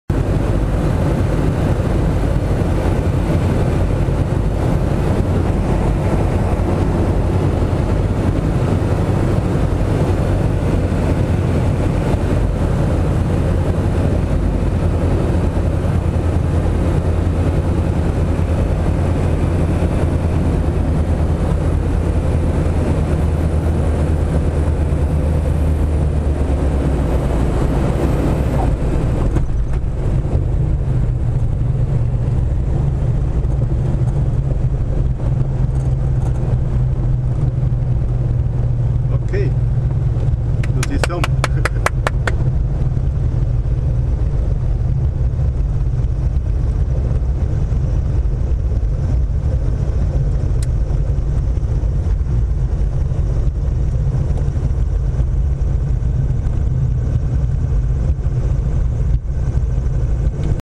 Landing 26 05 2013 Marc Meyns
Crosswind 12 knots, Runway 26, EBOS